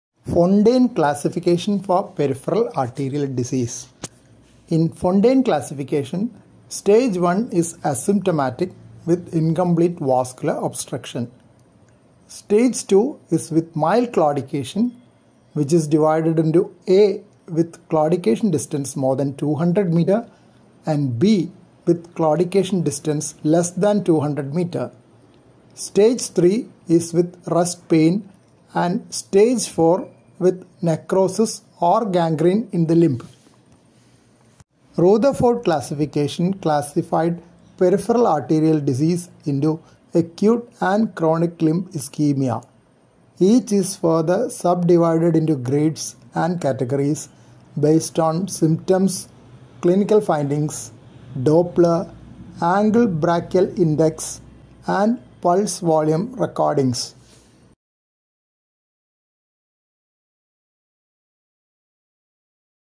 Fontaine classification for peripheral arterial disease
In Fontaine classification, Stage I is asymptomatic with incomplete vascular obstruction. Stage II is with mild claudication which is divided into A with claudication distance more than 200 m and B with claudication distance less than 200 m. Stage III is with rest pain and stage IV with necrosis or gangrene in the limb.
Rutherford classification classified peripheral arterial disease into acute and chronic limb ischemia. Each is further subdivided into grades and categories based on symptoms, clinical findings, Doppler, ankle brachial index (ABI) and pulse volume recordings.